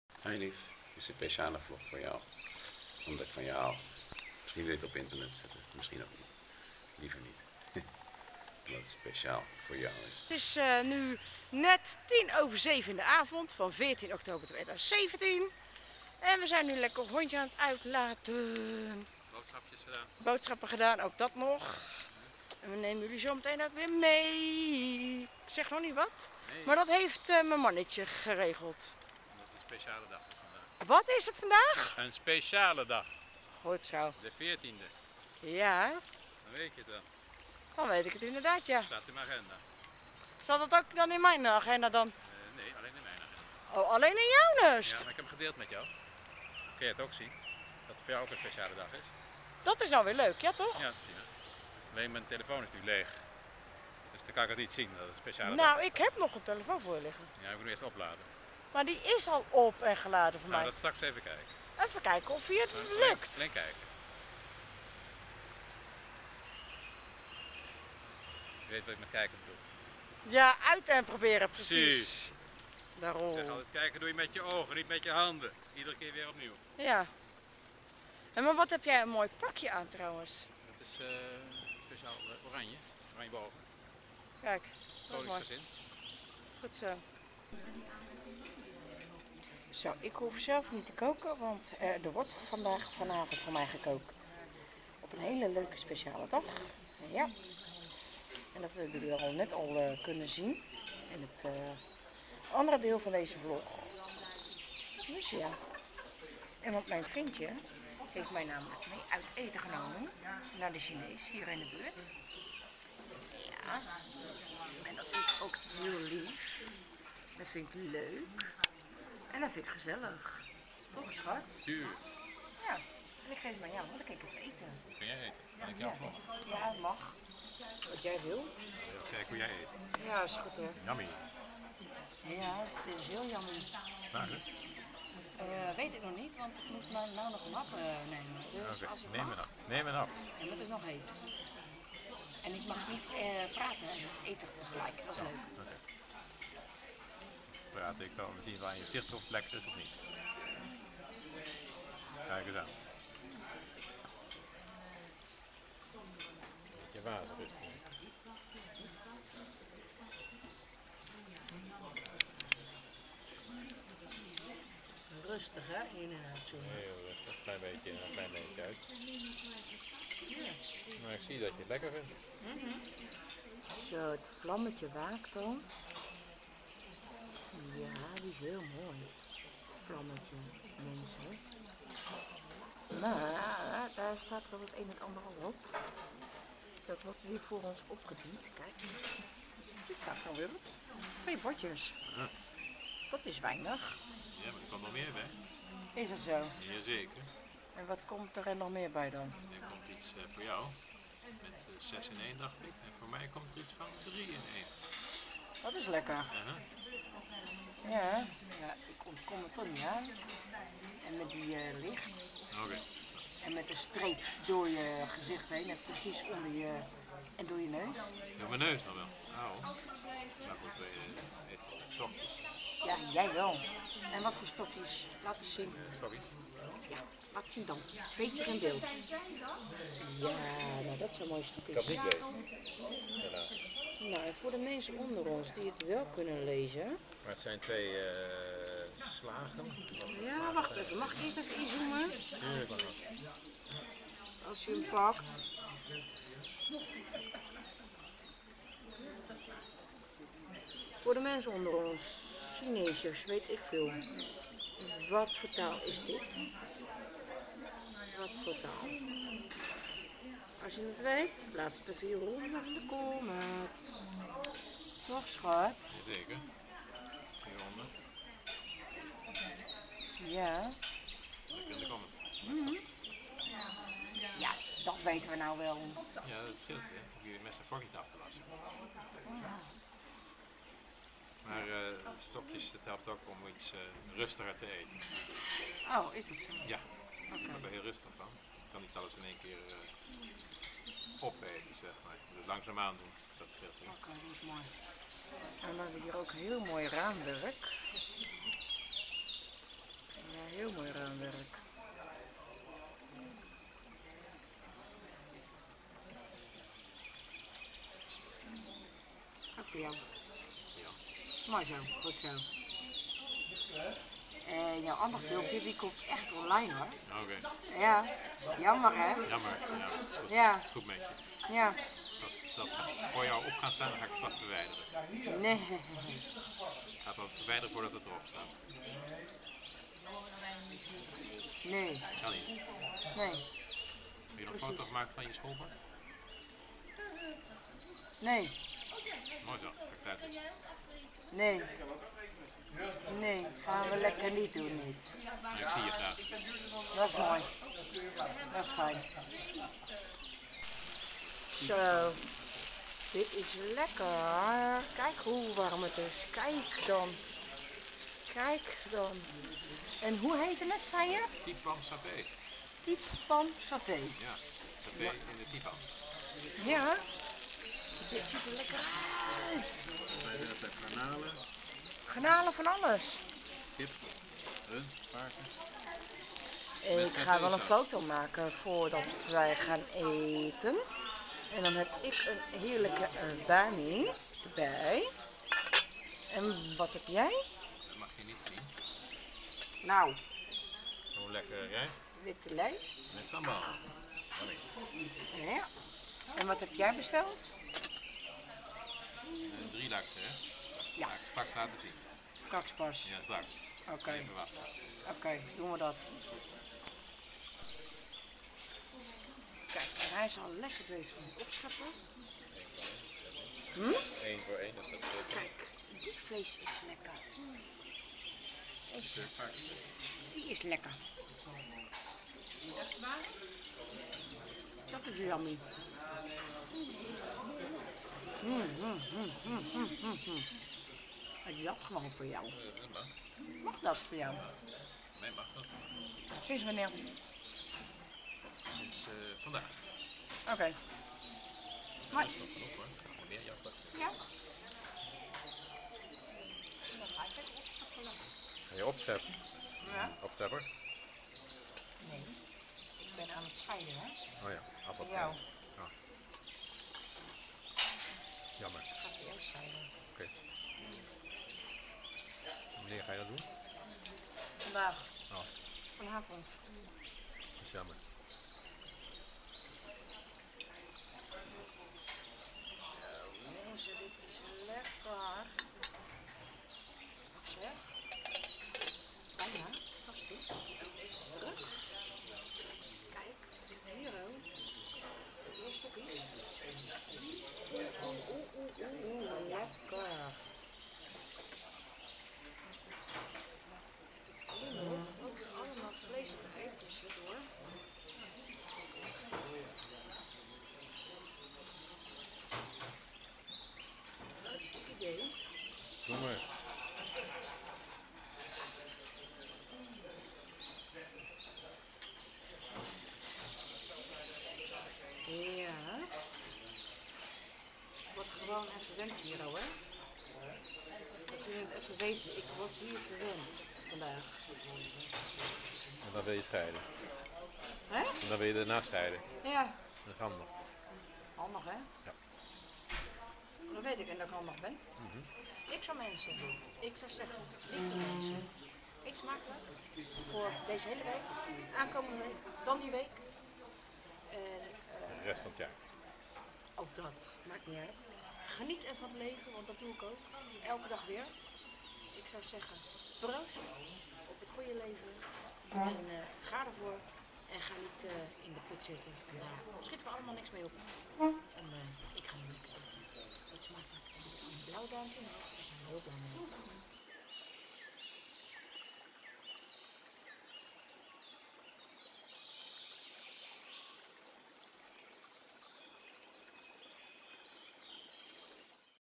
14-10-2017 verjaardag Maagverkleining 3 jaar #34
het is al het 3e jaar van mijn maagverkleining. Wat gaat de tijd hard zeg
Carin en Rod,
Een Wekelijkse vlog, we willen terug naar de dagelijkse vlog.